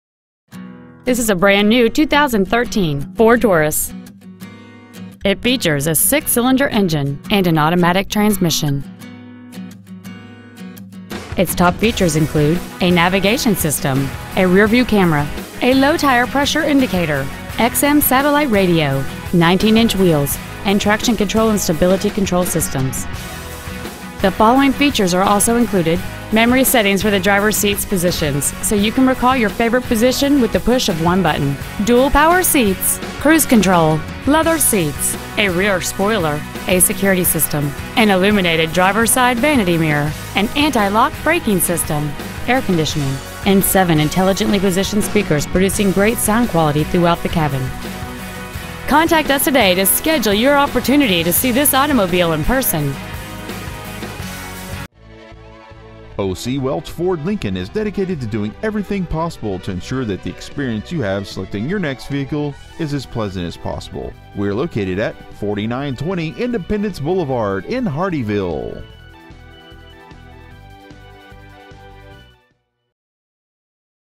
2013 FORD TAURUS Hardeeville SC
Year : 2013
 Make : FORD
 Model : TAURUS
 Engine : 3.5L V6 24V MPFI DOHC FLEXIBLE FUEL
 Trans . : 6-SPEED AUTOMATIC
 Exterior : TUXEDO BLACK METALLIC
 Miles : 6

 Stock : 00C53721